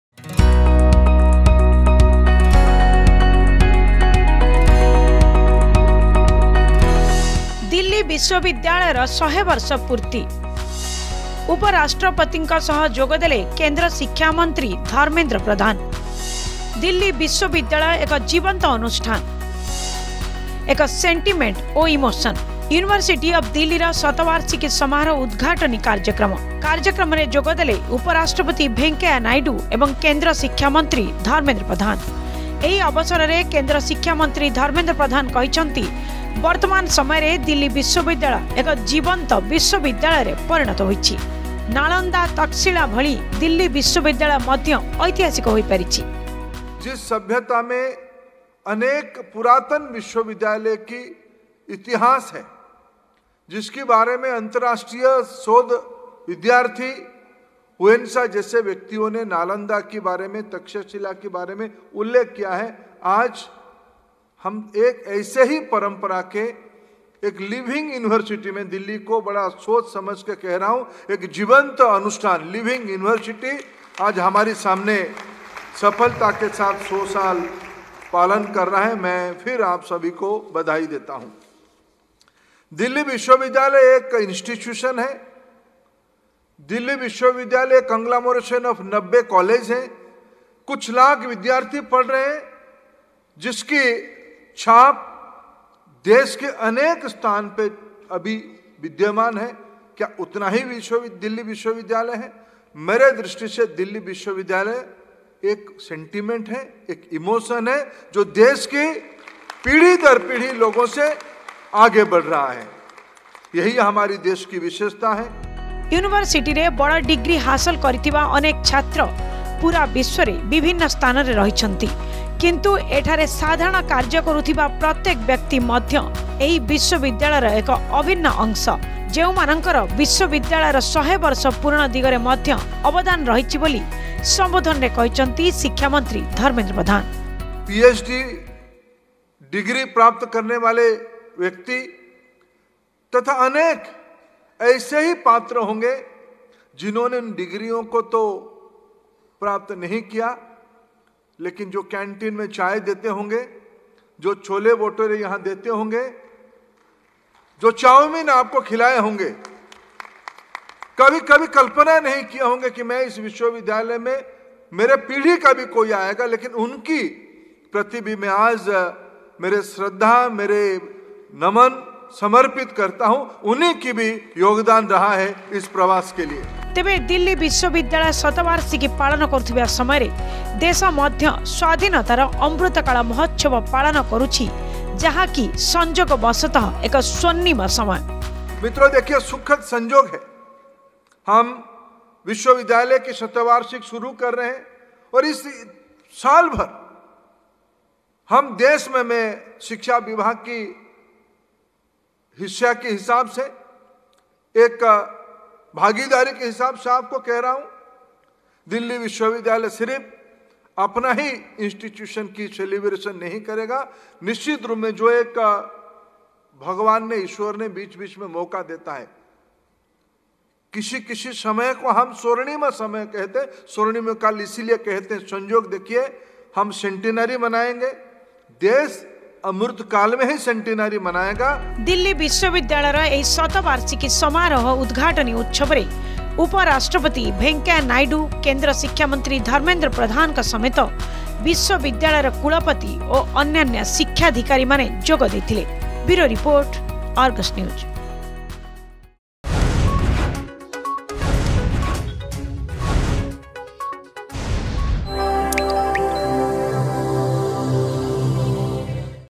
Delhi University's 100th Anniversary Addressed by Dharmendra Pradhan
On the occasion, Union Education Minister Dharmendra Pradhan said that Delhi University has now become a historic university. Like Nalanda and Takshashila, Delhi University has become a historic place.